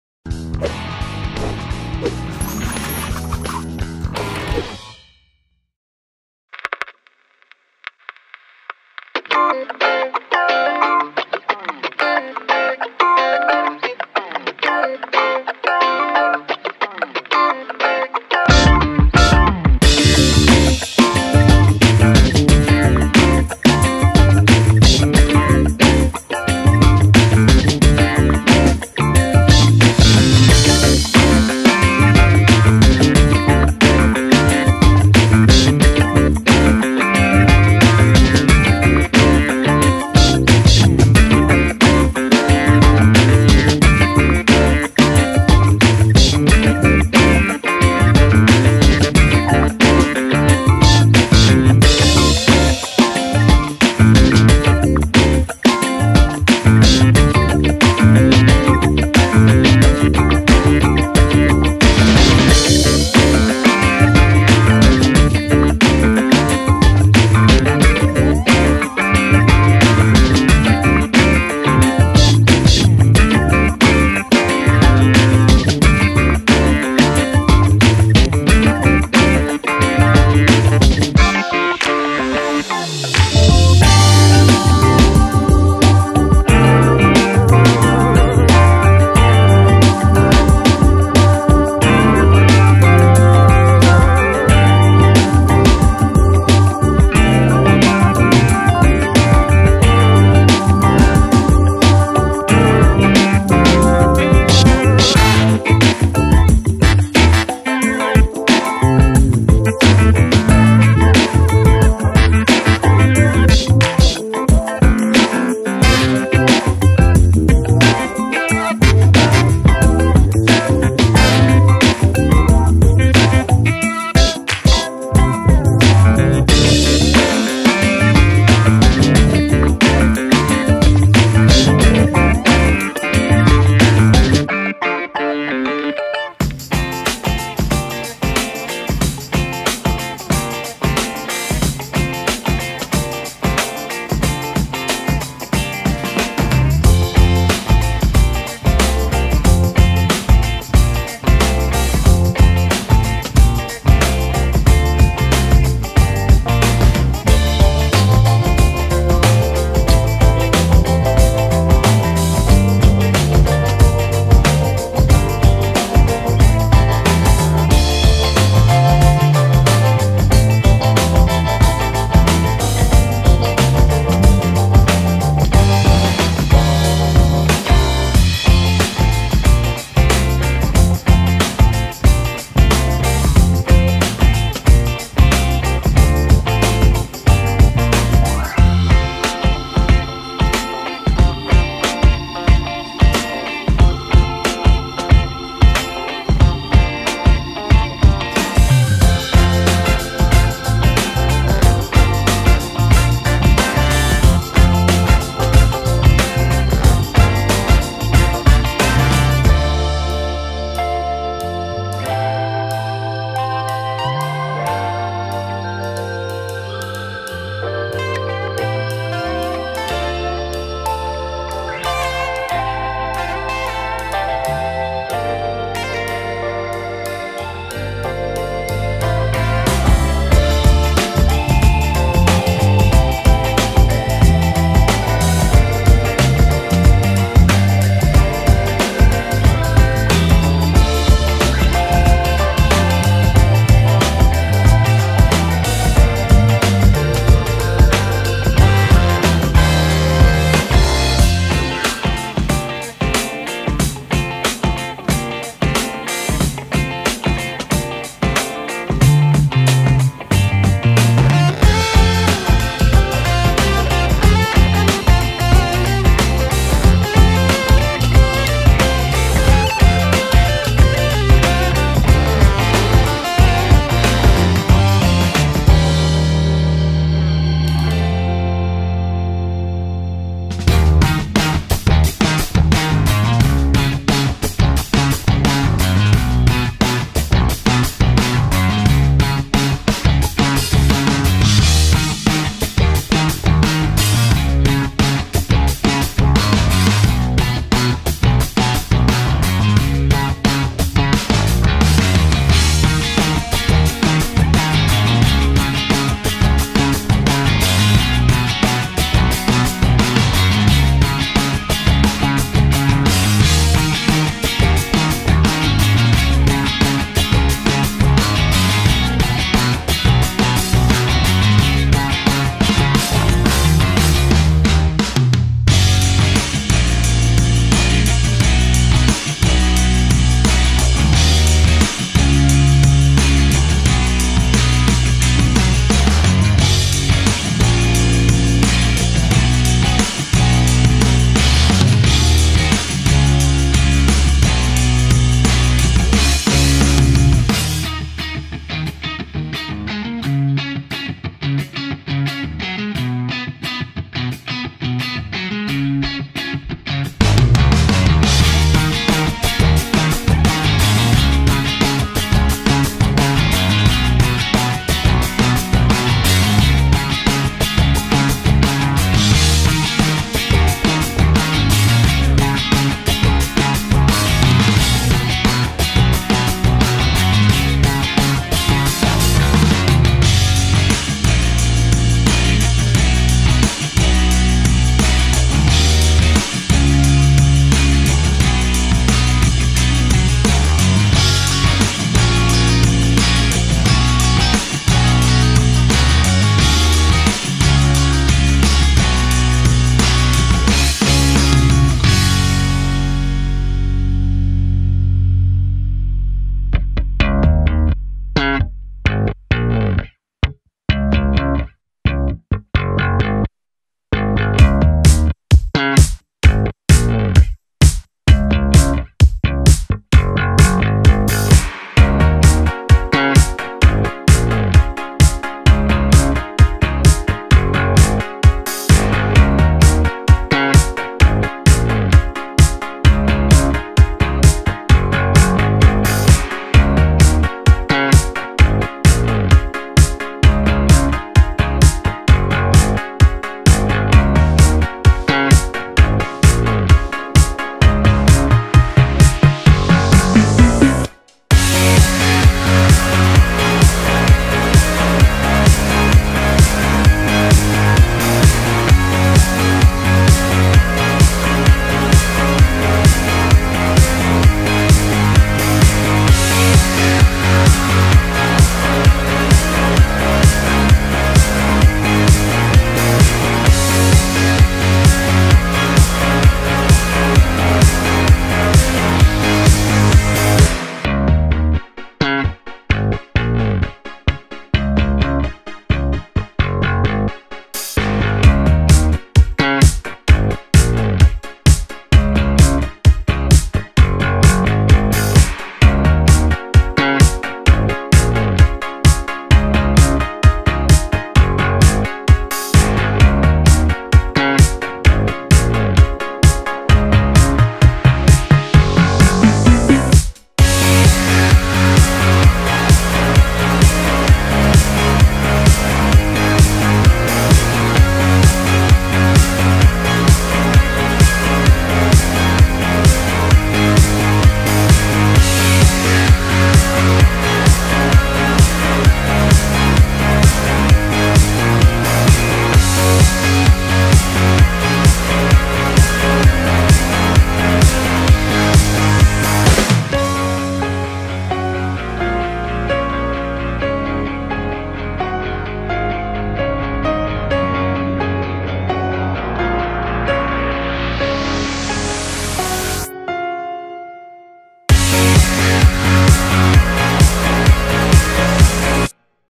29 ZEKİ EV VE TEMİZLİK TÜYOSU
Bu tüyolarımızla evlerinizi ışıl ışıl yapın

-Makyaj süngerinizi su dolu bir bardağa koyun, biraz deterjan damlatın ve mikrodalgaya atın.

-Kendi anti-bakteriyel mendillerinizi yapmak için limon suyu, su, sirke ve alkolü karıştırın.

-Kendi halı temizleyicinizi yapmak için bir bardak kabartma tozu, bir bardak mısır nişastası, birkaç karanfil, birkaç defne yaprağı ve bir kaşık boraksı blendıra atın. Sonra, karışımı halınızın üstüne serpin ve süpürün.

-Tuvaletinizin tıkanıklığını gidermek için üstünü streç filmle kaplayın ve bastırın, sonra da sifonu çekin.

Zaman bildirimleri:

0:43 – Makyaj süngerinizi nasıl temizleyebilirsiniz

2:22 – KY temizlik malzemesi

4:41 – İnanılmaz sünger tüyoları

6:47 – Tuvaletinizin tıkanıklığını nasıl açarısınız

8:02 – KY temizlik terliği